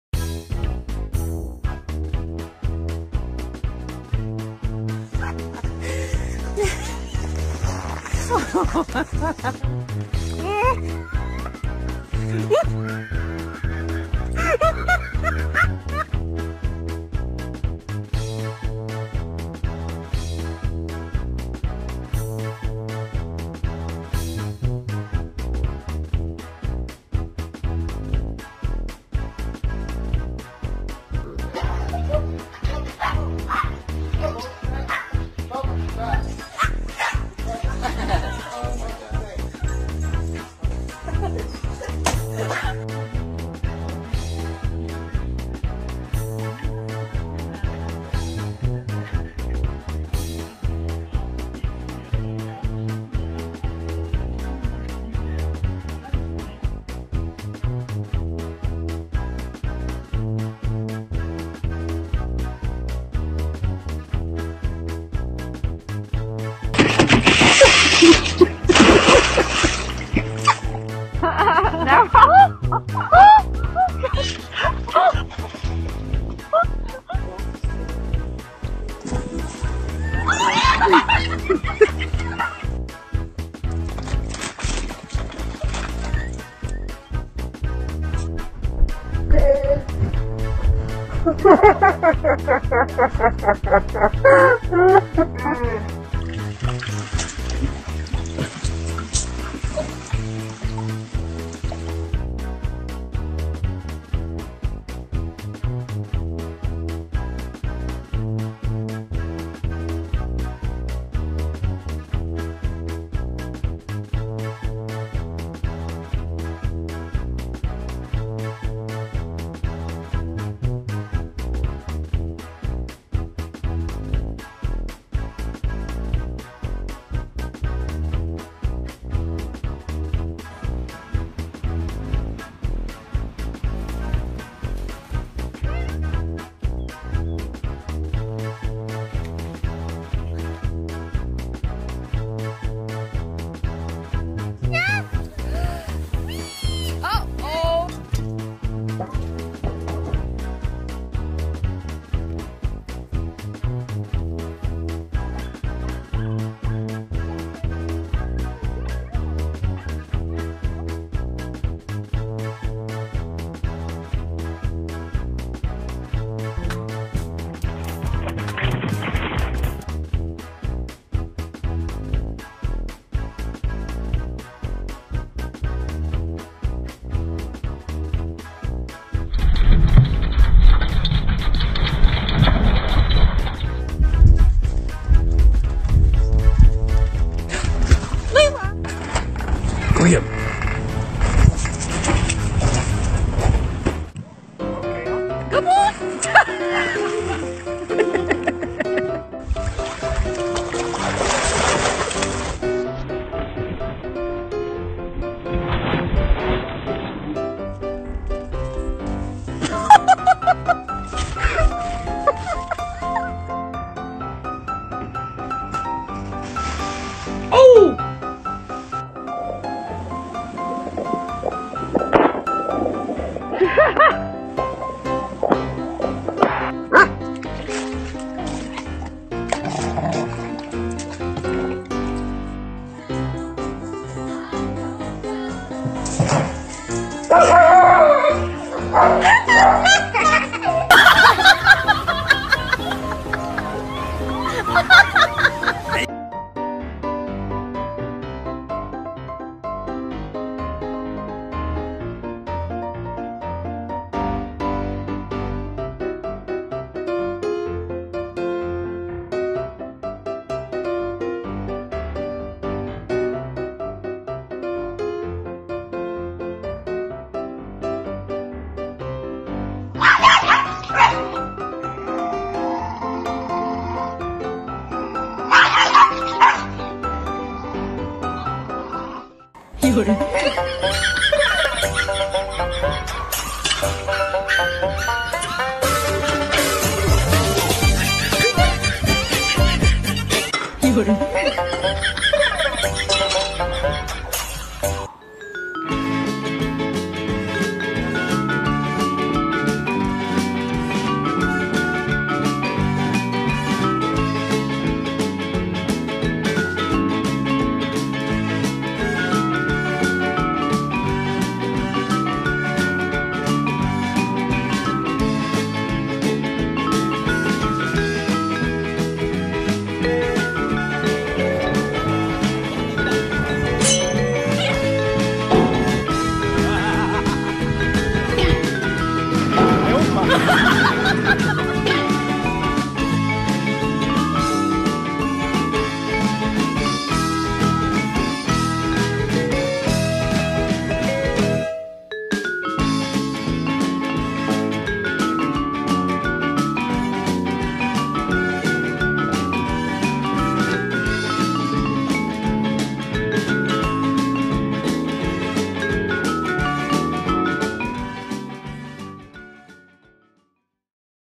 Try Not To Laugh 😜Funniest Animal Compilation🤣 Funny Dogs
Enjoy new funniest and very cute compilation of the week about try not laugh funny animals' life video.
Dogs are surely the most popular pets and awesome animals nowadays.
They are awesome animals. Despite all their differences, they have much in common, too.